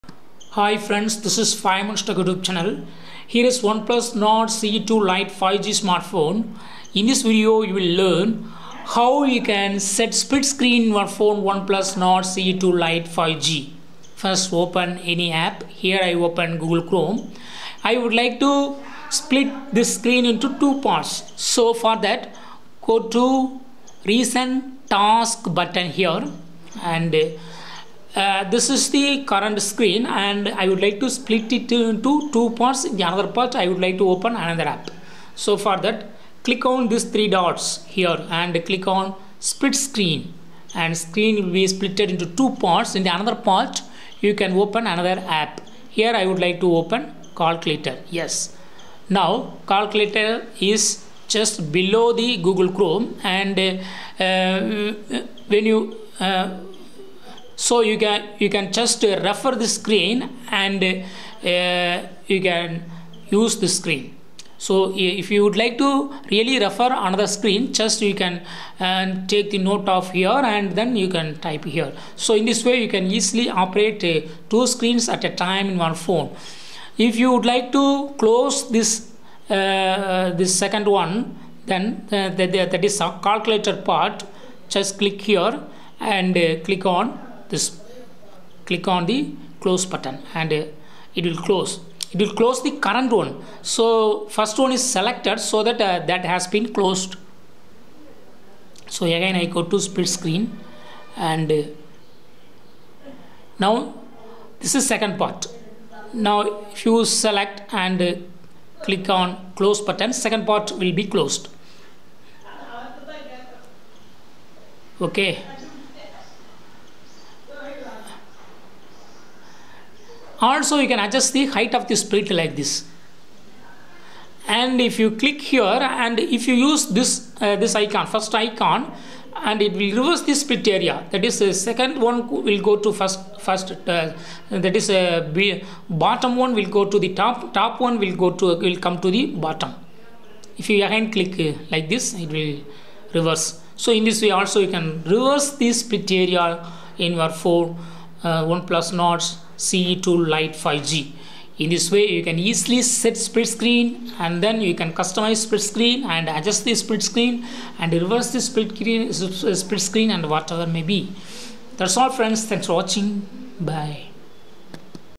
How to set split screen in Oneplus Nord CE2 Lite 5G
Oneplus Nord CE2 Lite 5G split screen settings: Learn here how to set split screen in Oneplus Nord CE2 Lite 5G smartphone